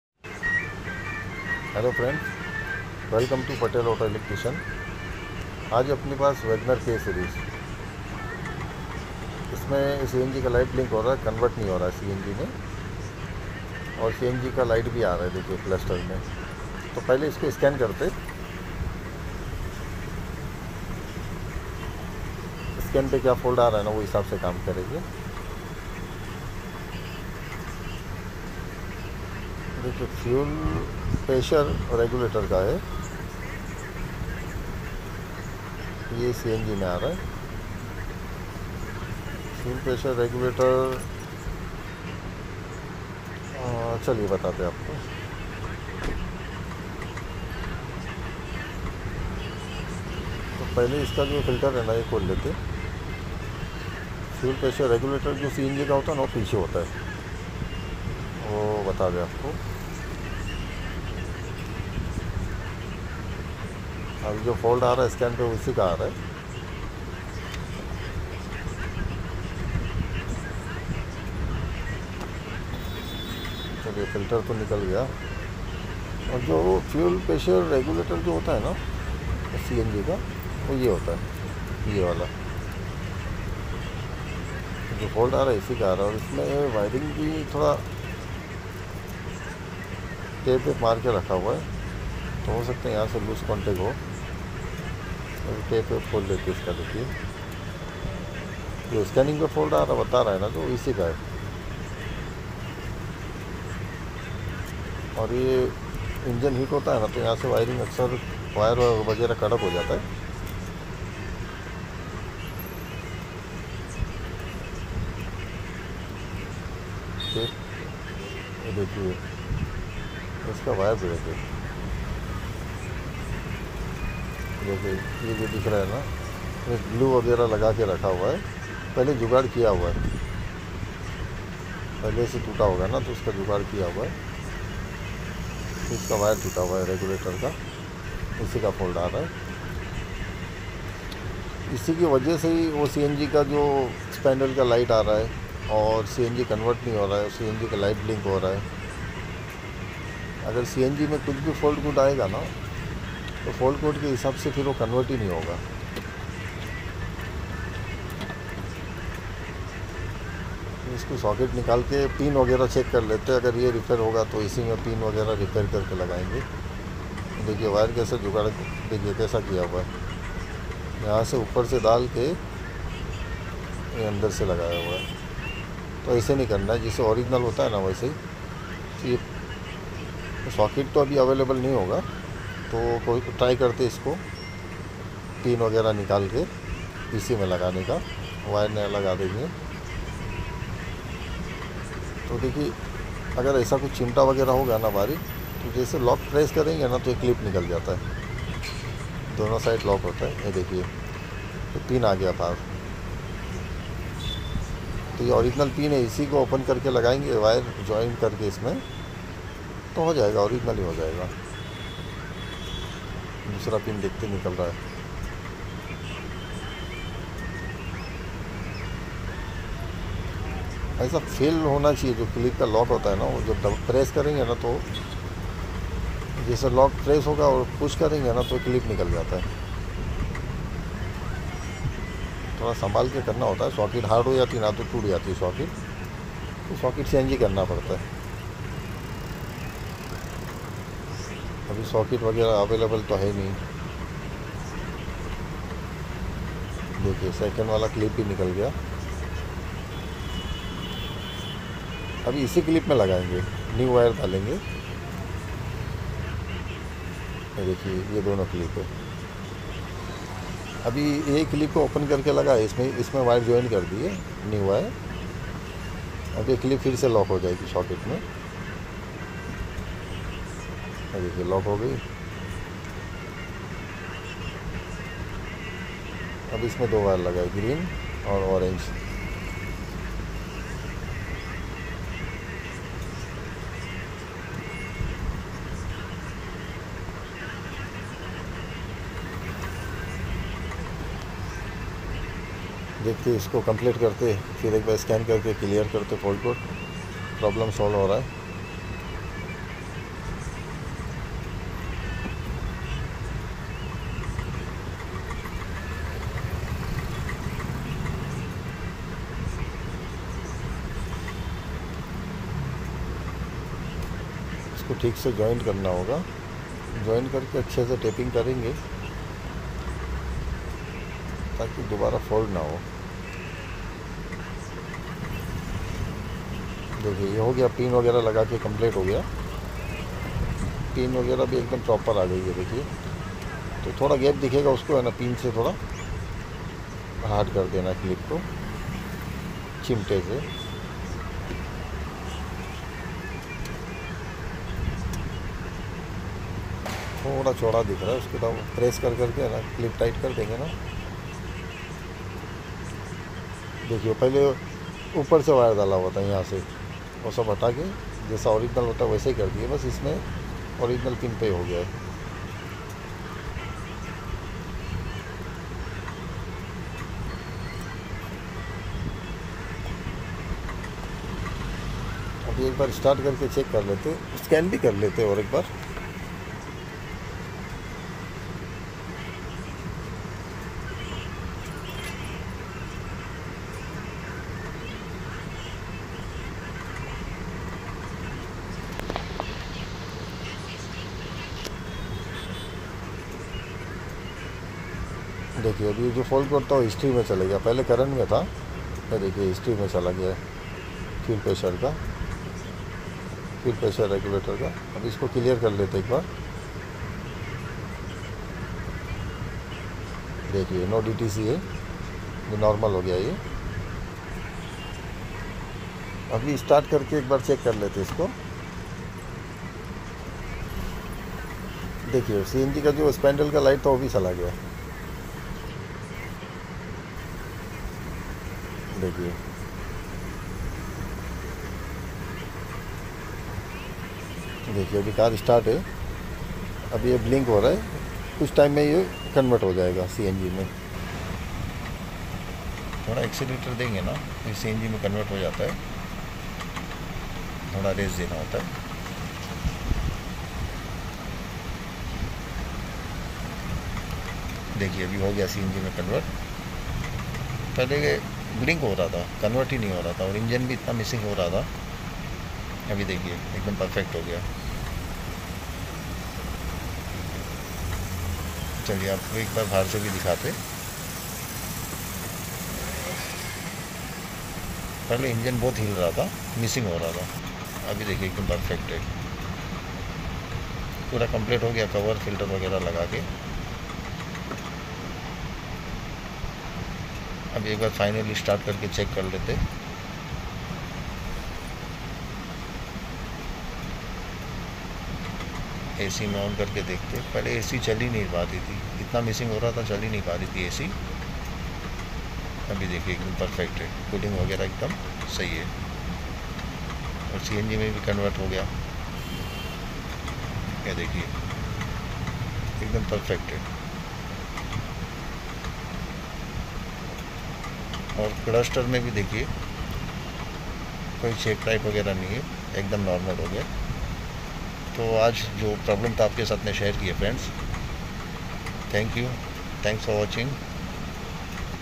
Maruti Suzuki Wagonr CNG convert problem and engine misfire
Friends, in this video we told you what to do if the engine in the car misfires and does not convert